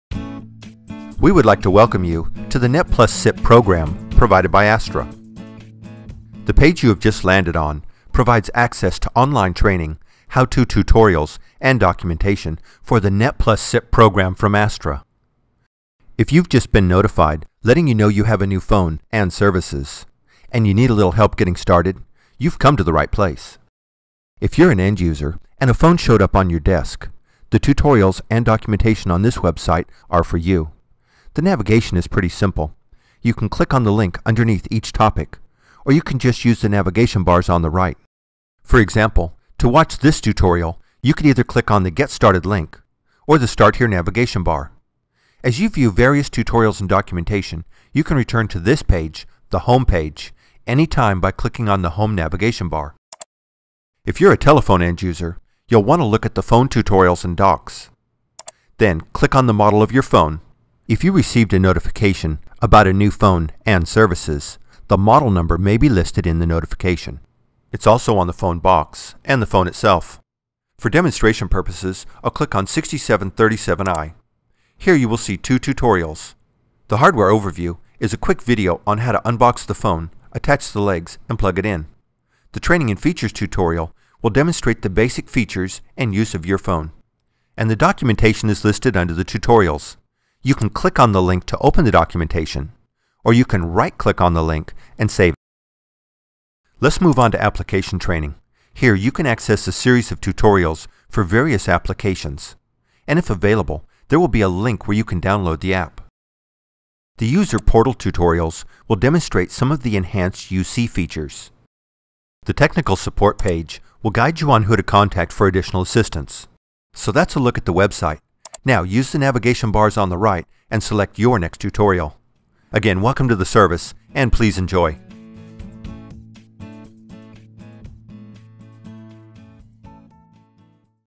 Mitel® / Internet2 Net+ SIP Program - Getting Started
This is a getting started guide and is intended to help new user navigate the Aastra / Internet2 Net+ SIP Program webpage and tutorials.